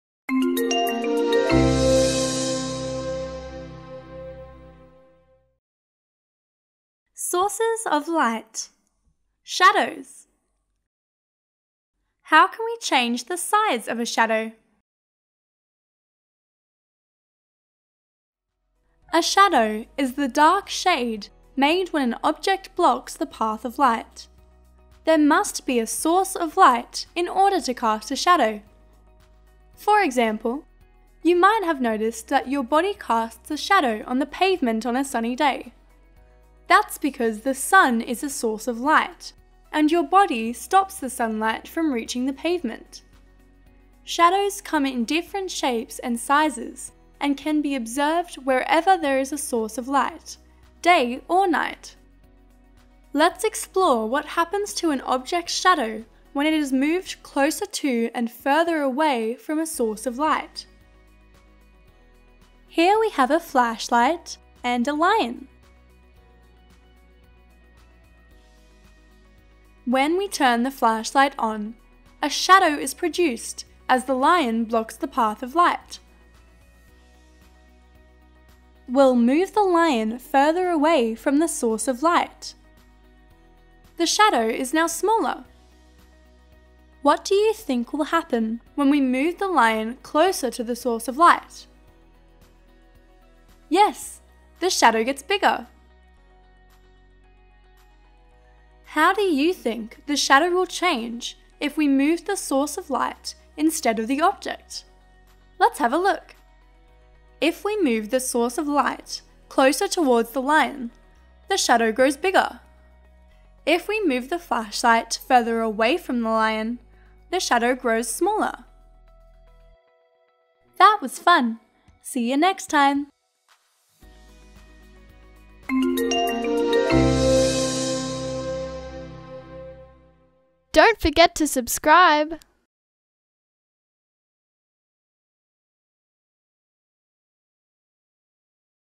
Changing the Size of Shadows
How does changing the positions of a light source and object affect the shadow that forms? Let's find out!

A shadow is a dark area that forms when an object blocks light.

You have learned that different materials can have different properties in terms of transparency.

Objects that are transparent allow almost all light to pass through them. They do not block light and no shadow is formed.

Translucent and opaque objects block light. When light hits translucent and opaque objects, a shadow forms. As translucent objects allow some light to pass through them, they generally form lighter shadows than opaque objects.

If you stand in sunlight on a clear day, you’ll notice that your body blocks some of the light from the Sun and a shadow forms on the ground. How does your shadow change throughout the day?

At sunrise, sunlight hits your body at a low angle and the shadow that forms is at its longest. As the Sun rises, the angle at which sunlight hits you increases and the shadow gets shorter. It will be its shortest when the Sun is directly above you – at around midday. It then gets longer as the Sun begins to set and will be at its longest again at sunset.

The distance between the light source and the object blocking the light also affects the shadow that forms. The closer the light source is to the object, the larger the shadow.